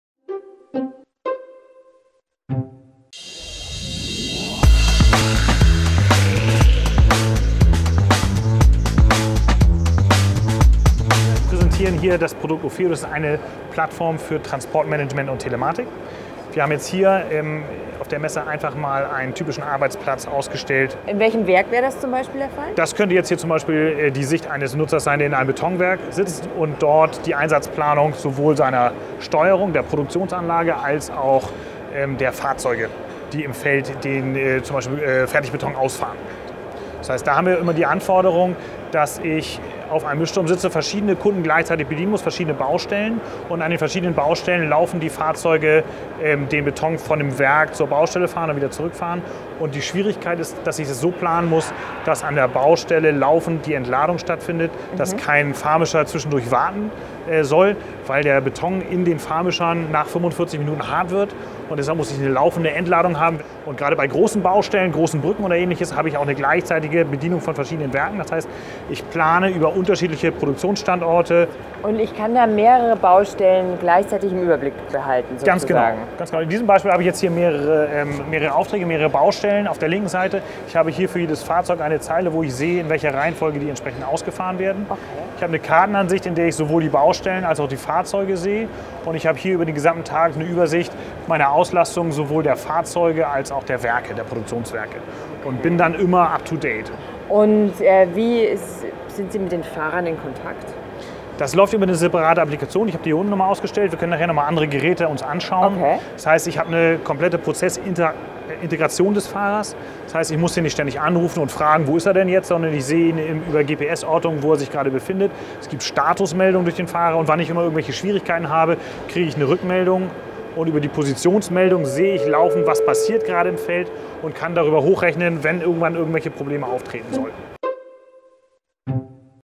Initions AG • OPHEO Software für Transportmanagement und Telematik
Die OPHEO Software der Initions AG zeigt detailliert die Tourenplanung und Abläufe im Transportmanagement.

Gerade bei großen Baustellen muss der Überblick über mehrere Aufträge gewahrt bleiben. Um effizient ausgelastet zu sein und um die Materialien rechtzeitig abzuladen. Gerade Beton würde nach 45 Minuten erhärten, daher ist die Zeitplanung welche die gesamte Transportkette einschließt sehr wichtig.

Interview mit Helge Plehn auf der bauma 2016.